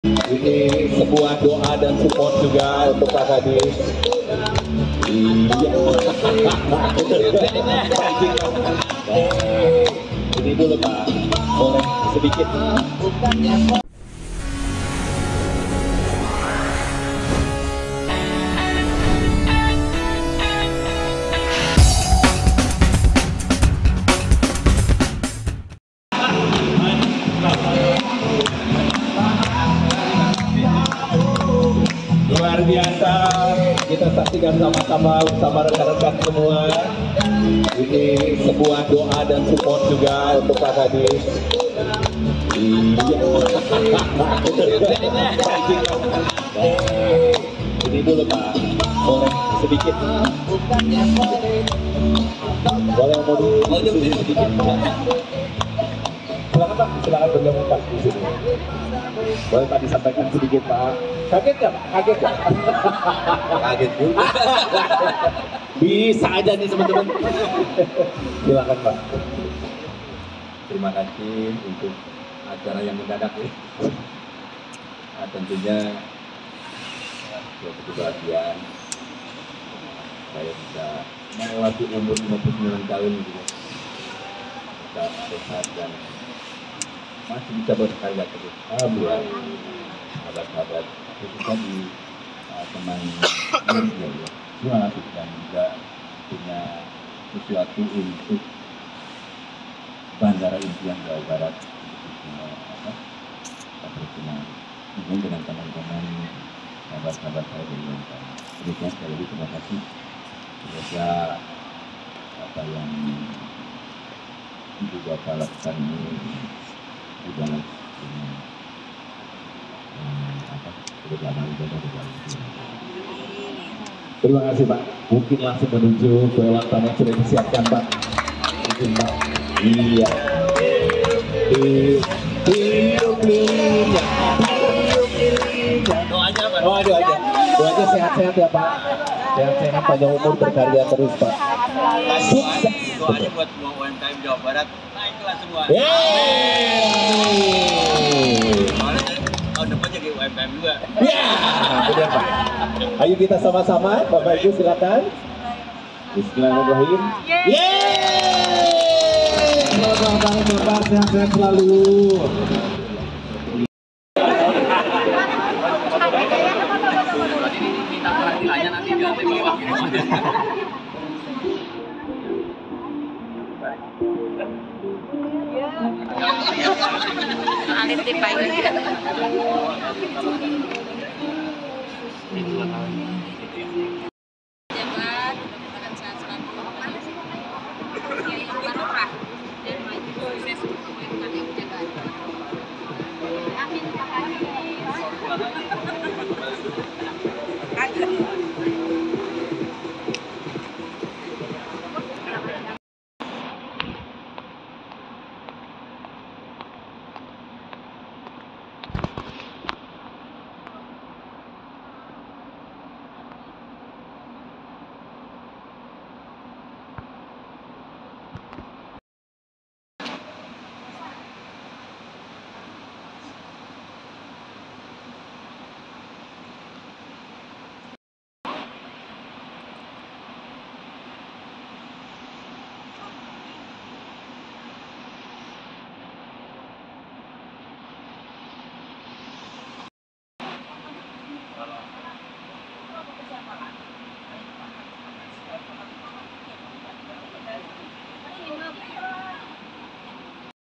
Surprise Ulang Tahun Kadis Diskuk JABAR
Selamat ulang tahun diskuk Jabar semoga panjang umur dan sehat selalu